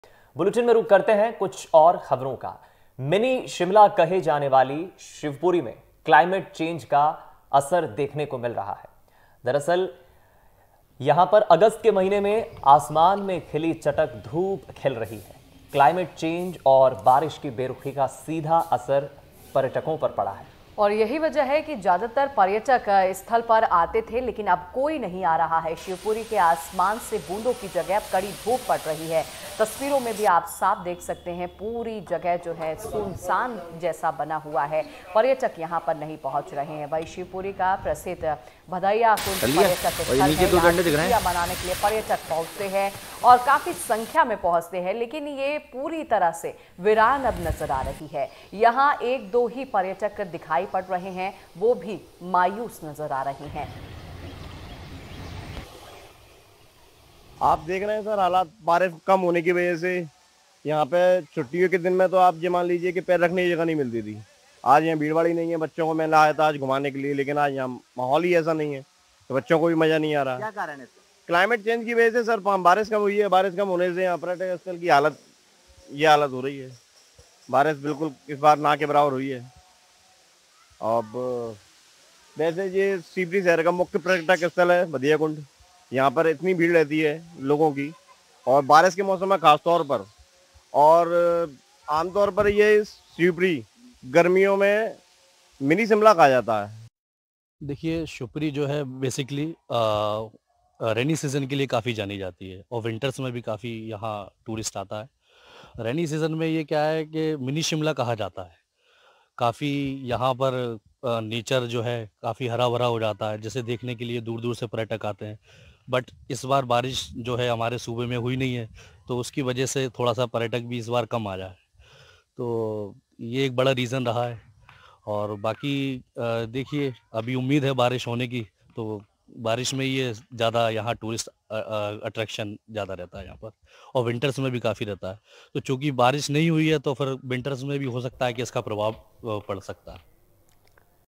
Madhya Pradesh का Mini Shimla के नाम से मशहुर Shivpuri क्यों पड़ा है वीरान? | MP News
मध्य प्रदेश (Madhya Pradesh) शिवपुरी (Shivpuri) को मिनी शिमला (Mini Shimla) कहा जाता है।  शिवपुरी (Shivpuri) में भी क्लाइमेट चेंज (Climate Change) का असर देखने को मिल रहा है। सभी पर्यटक स्थल खाली पड़े हुए है।